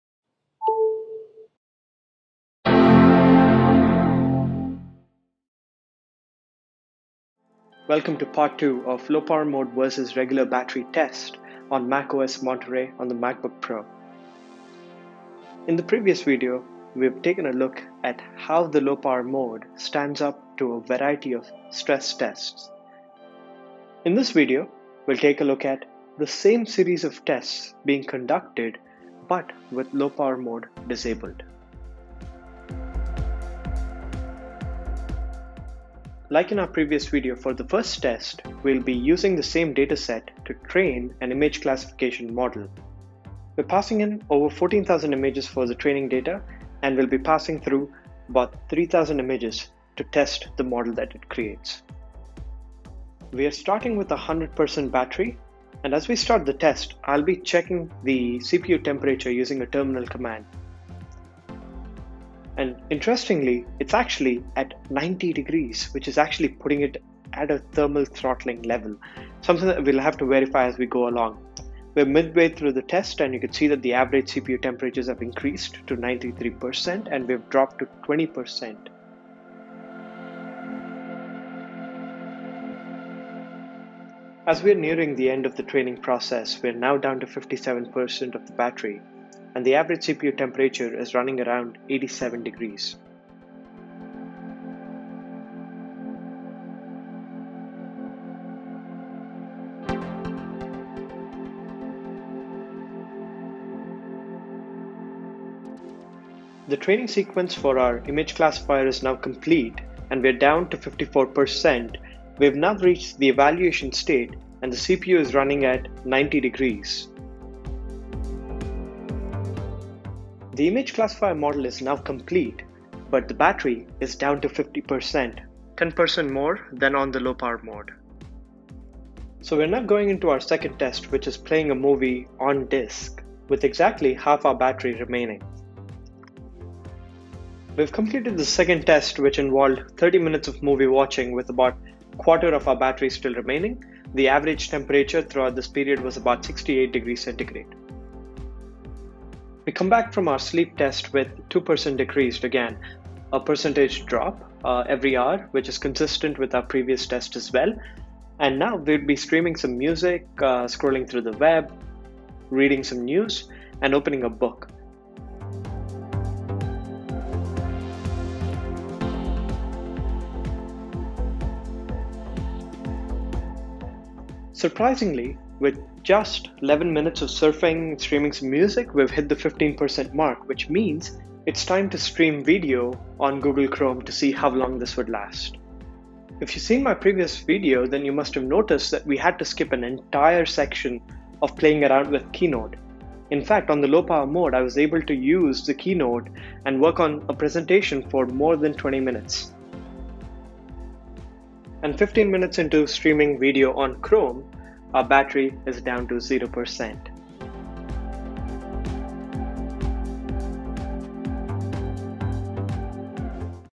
Regular vs Low Power Mode Battery Stress Test Intel MacBook Pro Part-2 || macOS Monterey 12.0.1
Hello Viewers, and welcome to Part - 2 of the Battery & Efficiency test on the 2019 Intel MacBook Pro running macOS Monterey. With the release of macOS Monterey, Apple's bought the Low Power Mode to the Mac. According to Apple, Low Power Mode on Mac reduces the system clock speed and the display brightness in order to extend your battery life even further.

Now that I’ve had a chance to play around with macOS Monterey, I was curious to see if The Low Power Mode had a significant impact on my MacBook Pro - and if yes, then how much. In the last video, I put my personal MacBook Pro through a stress test, checking at the battery drain in intervals, overall performance and temperature. In this video we'll take a look at how it holds up to the same series of tests in regular mode.

Keep in mind that this is not a scientific way to test the battery performance but should give us a good idea to gauge the overall performance difference.

MacBook Pro Specs: (used in this test)
MacBook Pro (13-inch, 2019, Four Thunderbolt 3 ports)
Processor: 2.4 GHz Quad-Core Intel i5
Memory: 8 GB
Graphics: Intel Iris Plus Graphics

Tests Performed in Order of Operation:
1. Creating an Image Classification Model using CreateML App,
2. Loading an on-disc movie using VLC for 30 Min.,
3. 2 hour sleep test,
4. Web browsing + Streaming Music online + Book reading simulation
5. Working on Keynote
6. Video streaming using Google Chrome

**Throughout the video, I have the Quicktime Screen Recording Enabled & Running

Let's R YouTube Channel Link: (For Data Science and Business Analytics)

In the next video of this series, I'll be giving out a side by side comparison and my observations on the overall performance of the notebook.

It’s free and will help out more than you think.